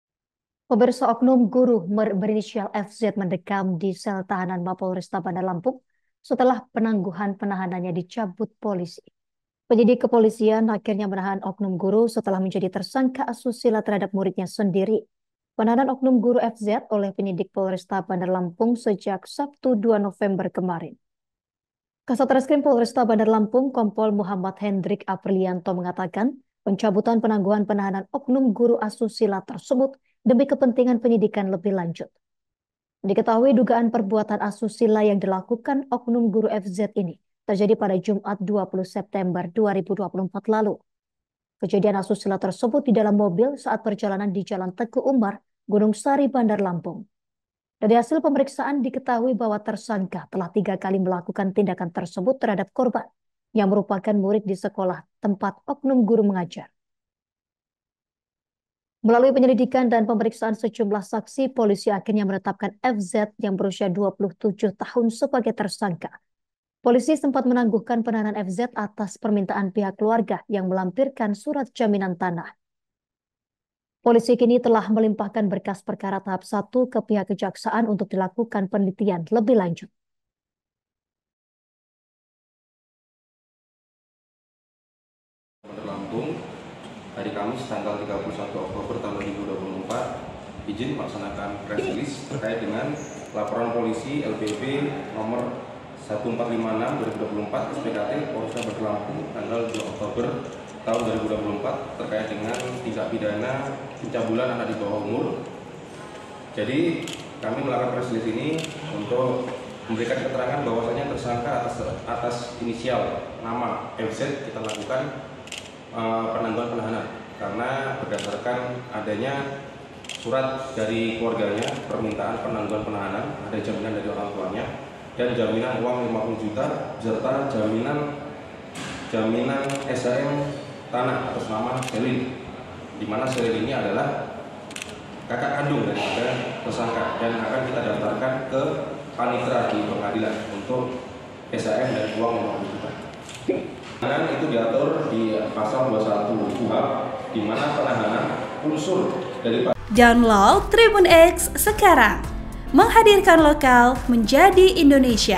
Guru SD Cabul Bebas seusai Serahkan Uang Rp 50 Juta & Sertifikat Tanah, Kembali Ditahan
Laporan wartawan Tribun Lampung - Hurri Agusto
TRIBUN-VIDEO.COM- Oknum guru berinisial FZ mendekam di sel tahanan Mapolresta Bandar Lampung setelah penangguhan penahanannya dicabut polisi.

Penyidik kepolisian akhirnya menahan oknum guru setelah menjadi tersangka asusila terhadap muridnya sendiri.

Penahanan oknum guru FZ oleh penyidik Polresta Bandar Lampung sejak Sabut (2/11/2024) kemarin.(*)

Program: Live Update
Host: Yustina Kartika Gati
Editor Video : Rifqi Khusain
Uploader: Unzila AlifitriNabila